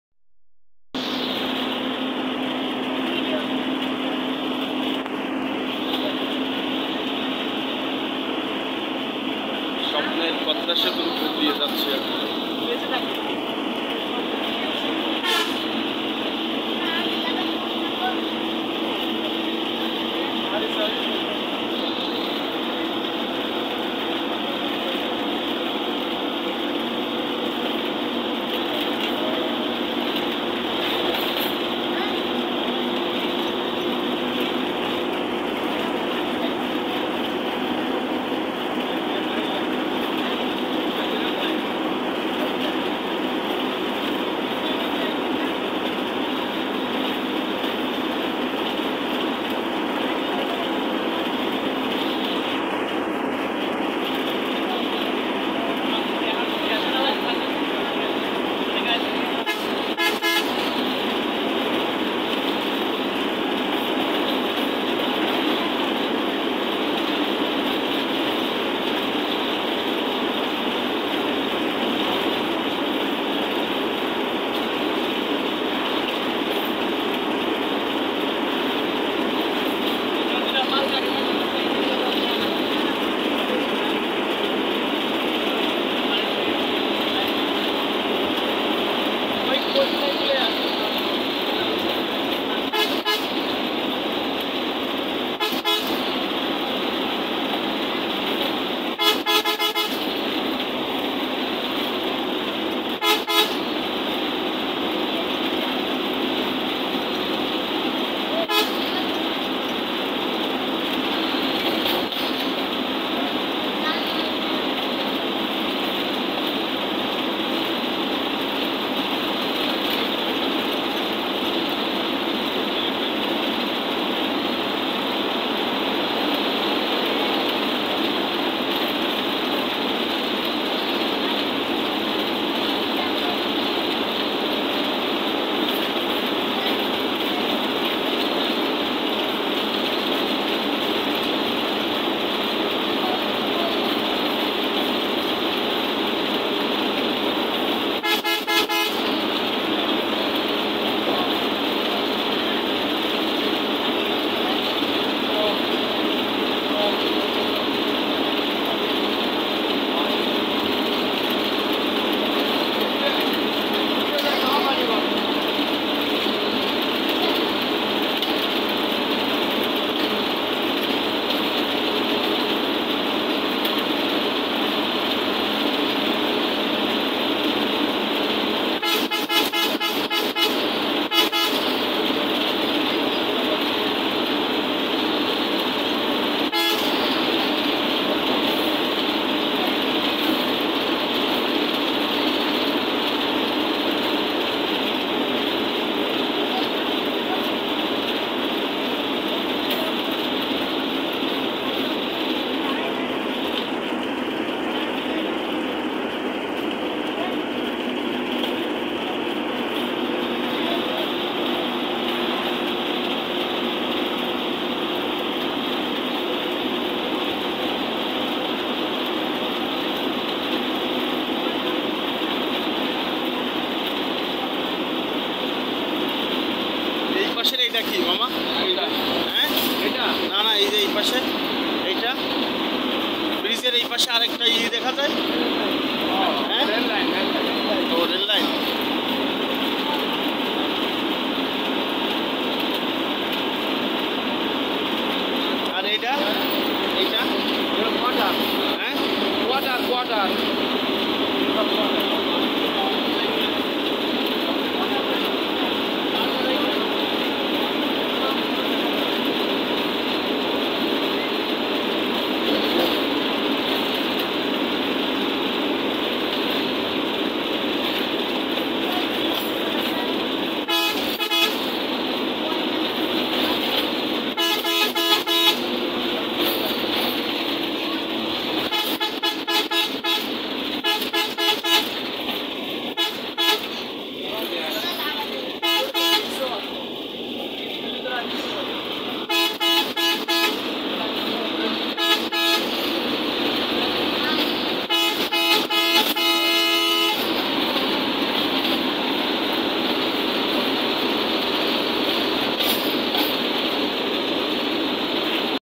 New in Dhaka স্বপ্নের পদ্মা সেতু
পদ্মা সেতু পার হয়ে গেলো আমার কাছে আলাদা ভাবে পরিচিতি পায় বাংলাদেশের ইতিহাস সম্বলিত একটি নদী।